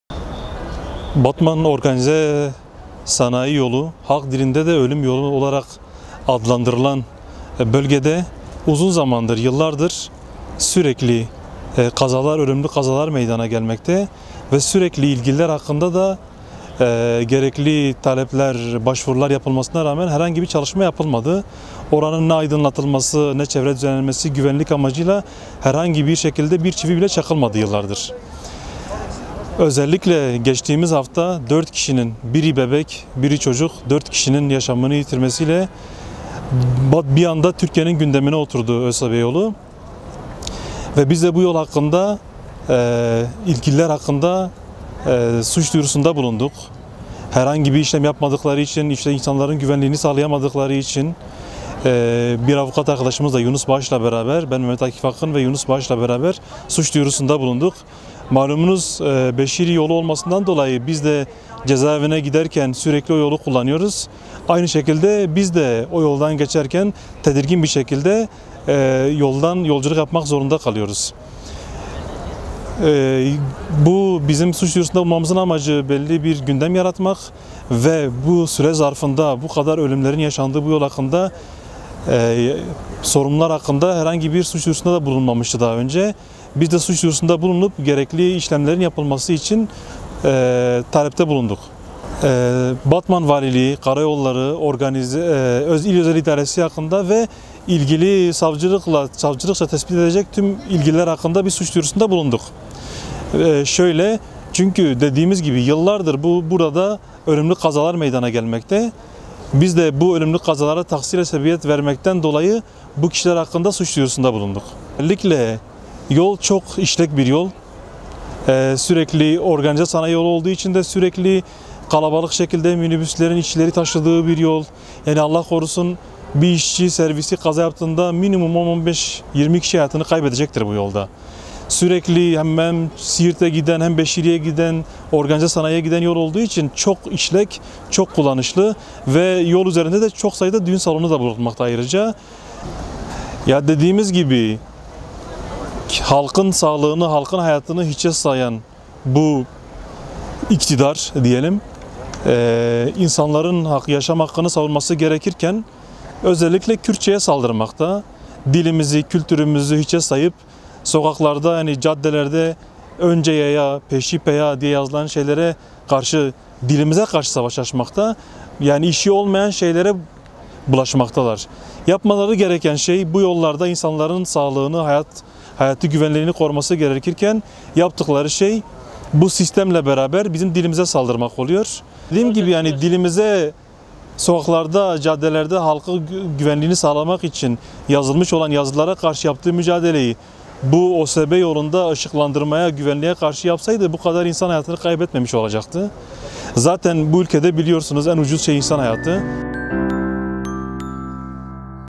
'ÖLÜM YOLU'NDA TEDBİRSİZLİK CAN ALIYOR
Êlih’te “Ölüm Yolu” olarak nitelendirilen yolda trafik lambası ve aydınlatma olmamasından kaynaklı ölümlü kazalar yaşanıyor. DEM Partili Zeynep Oduncu, "Kürtçeyle uğraşacağınıza önlem alın" dedi